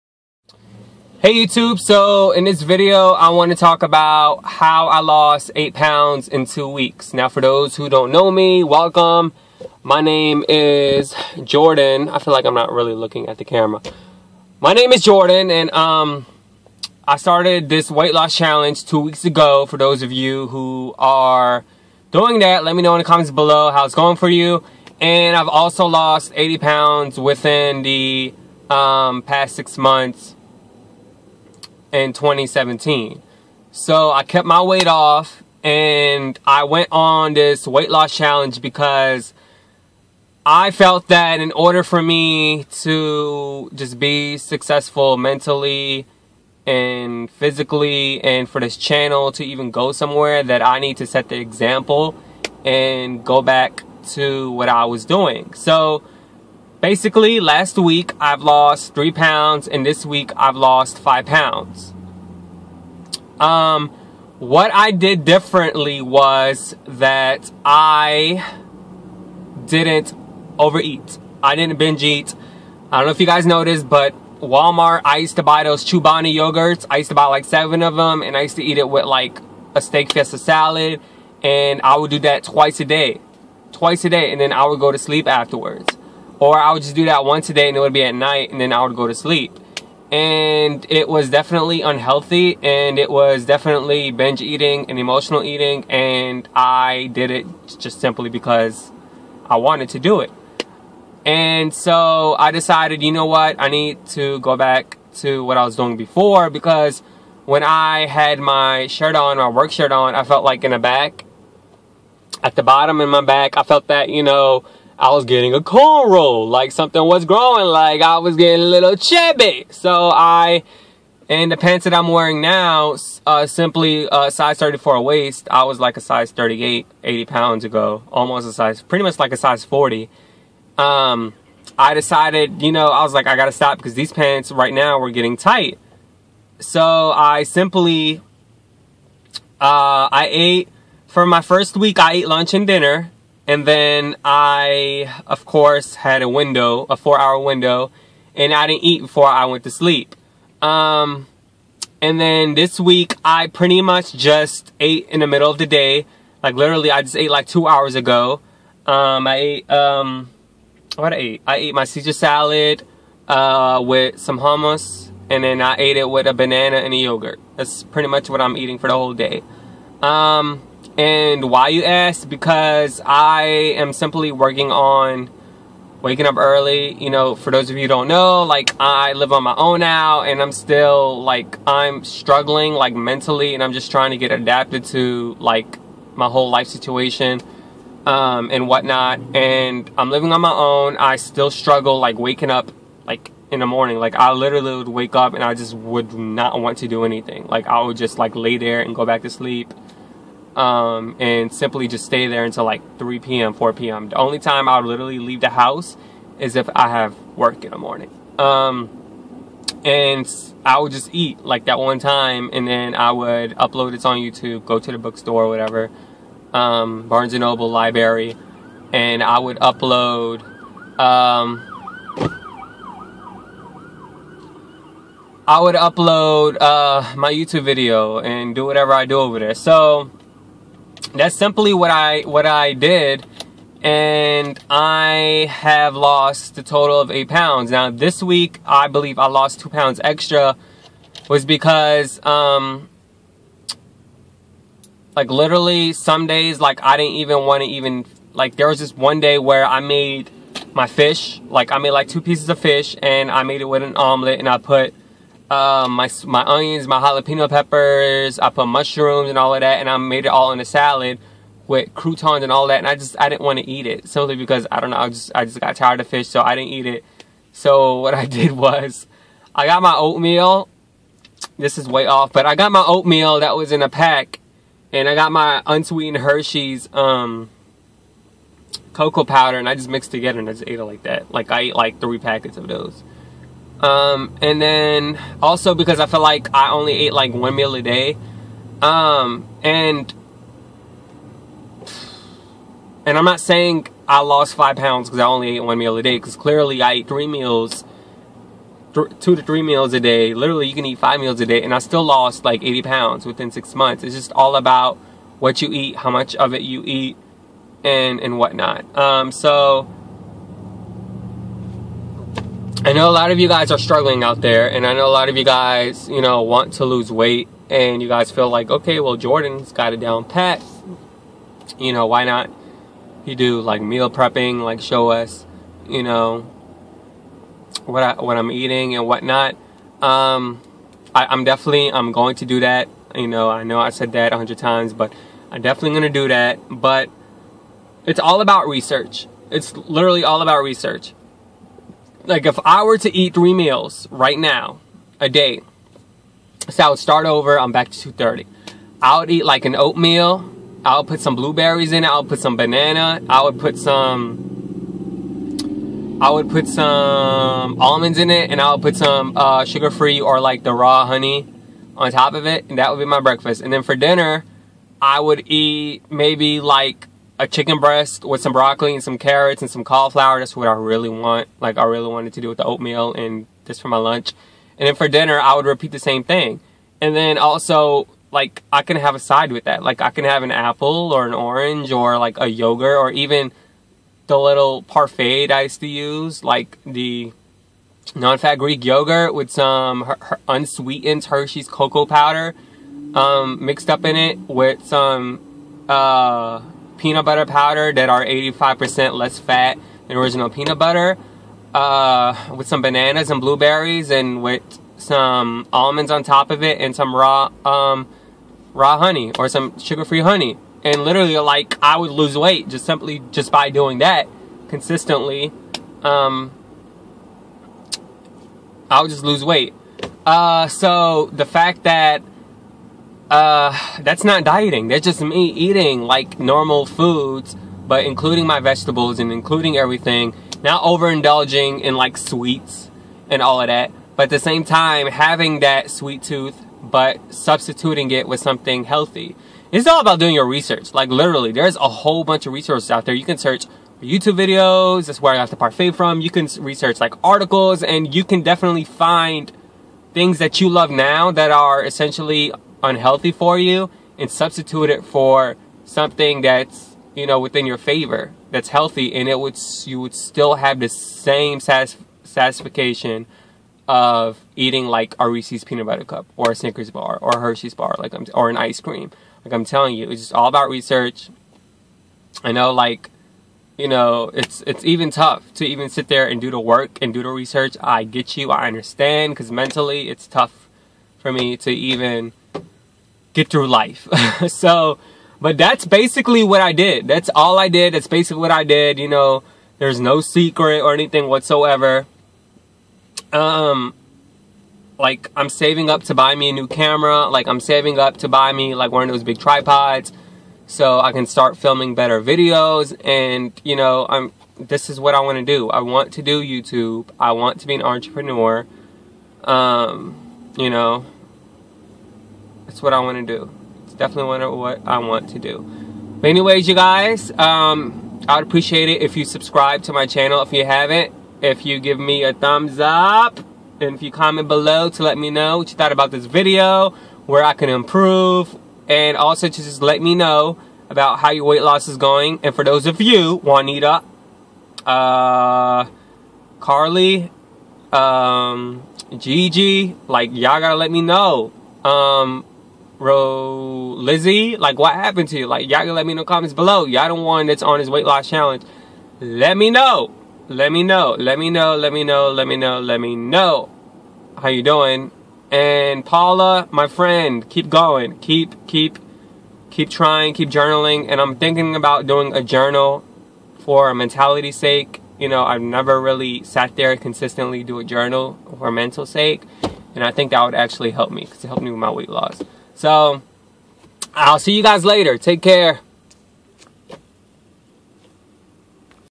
HOW I LOST 8LBS IN 2 WEEKS | WEIGHT LOSS UPDATE 2018
In this video, I talk about how I lost 8lbs in 2 weeks. Today is Friday, so it is my weight loss update.